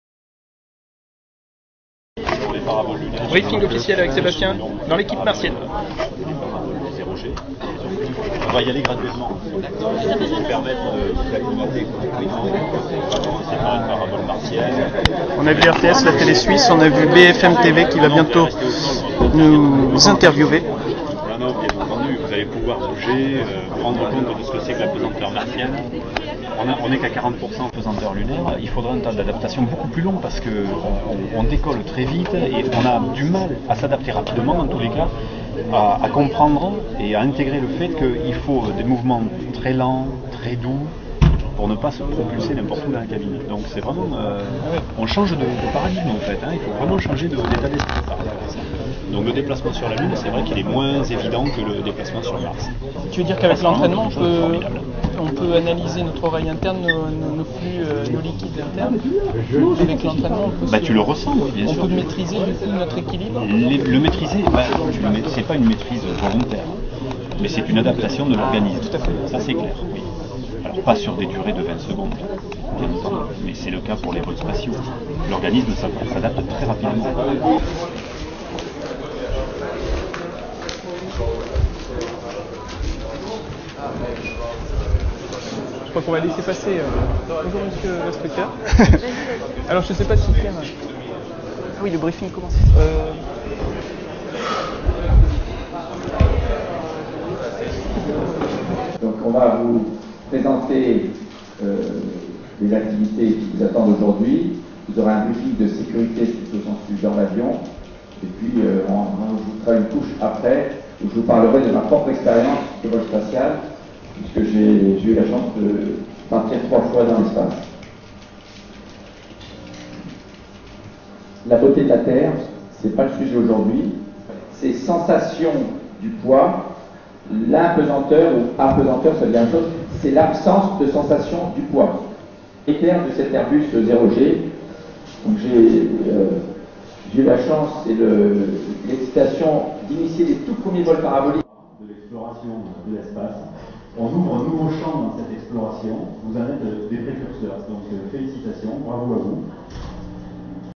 Vol Zéro G (3/7) : briefings des équipes avant le décollage
Peu avant 11 heures, après avoir enfilé sa combinaison et fait connaissance avec les membres de son équipe, Jonathan Beneteau notre reporter embarqué, C-YourMag.net by Cap Sciences et la quarantaine de passagers sont invités à écouter les derniers conseils des spationautes, dont Jean-François Clervoy qui comptabilise trois vols dans l'espace et Sébastien Rouquette, docteur en planétologie.

Vol Zéro G : revivez la zéro gravité avec Jonathan, reporter embarqué en cliquant sur le lien suivant

Images Jonathan Beneteau et Claire Sémavoine, montage Claire Sémavoine pour C-YourMag.net by Cap Sciences.